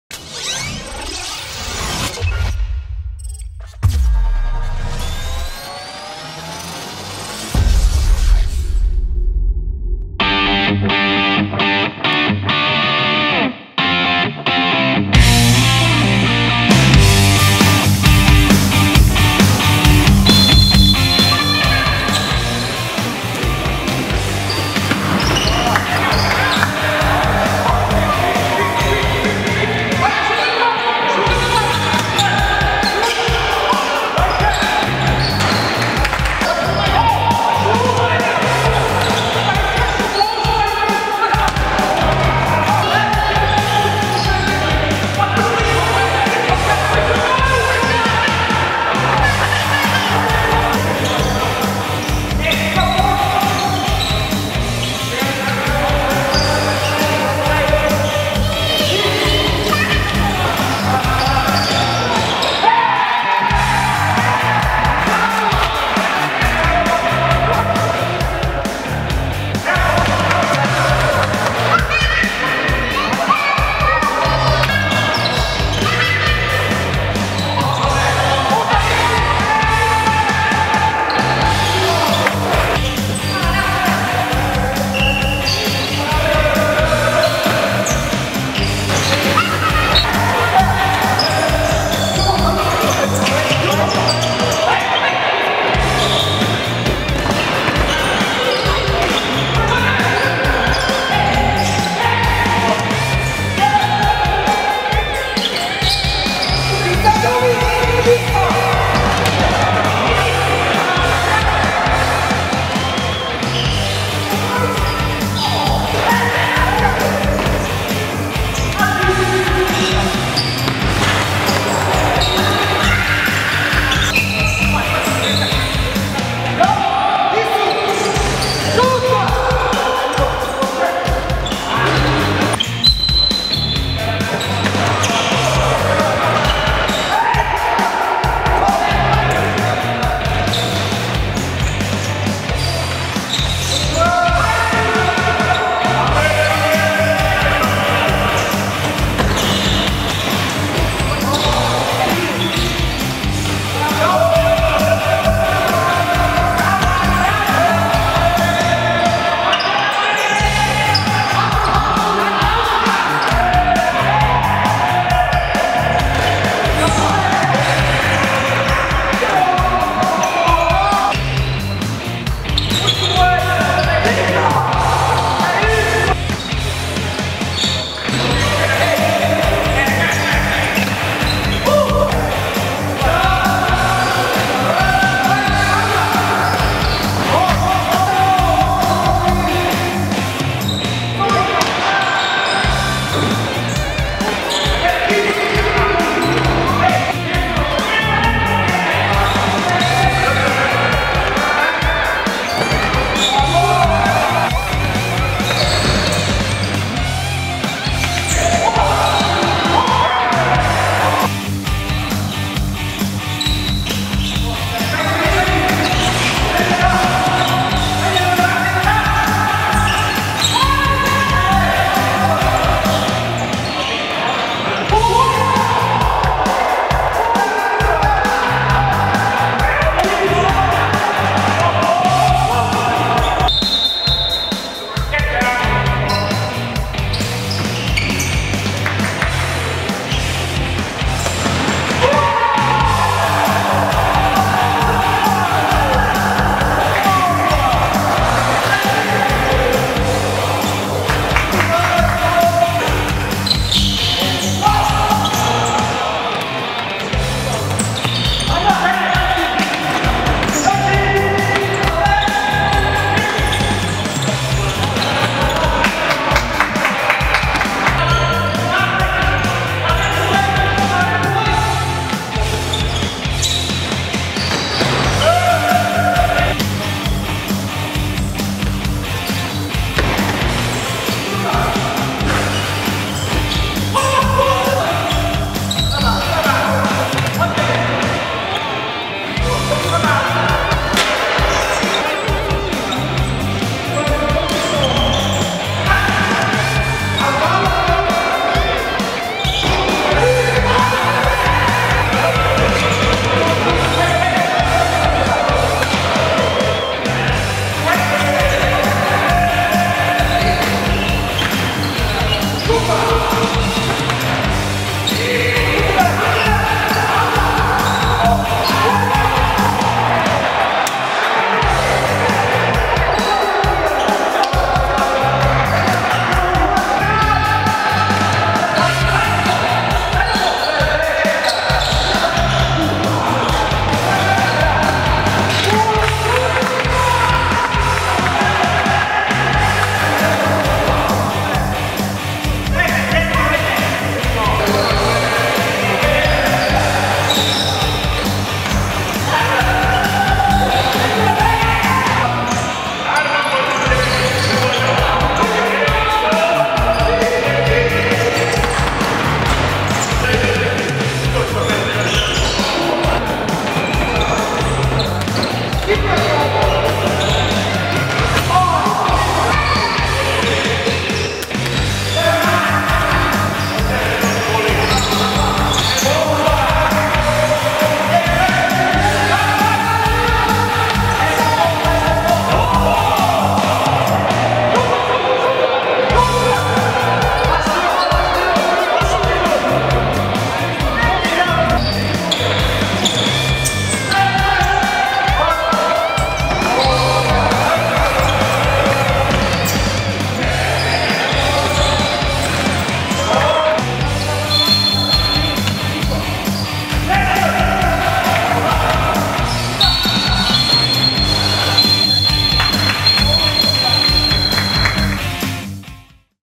Puskas & Hagi Futsal vs Academic Futsal (2:4) Emerald Futsal League Highlights
In the final game of week #9 in the Emerald Futsal League, Puskas & Hagi Futsal lost against Academic Futsal 2:4.

Goals for Puskas&Hagi Futsal:
Tamas Toth, Adrian Ponoran

Goals for Academic Futsal:
Joel Rocha x2, Jose Ferreira, Manuel Ramos

Video Production:
Silver Hat Marketing